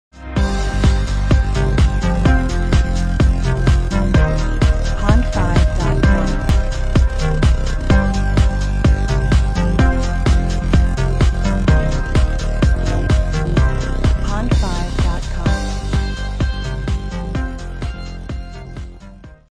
City Half Marathon Runners | HD Stock Video Footage | Production Music
This is very urban footage of marathon city runners, they have carts, and they are wearing sporty clothes.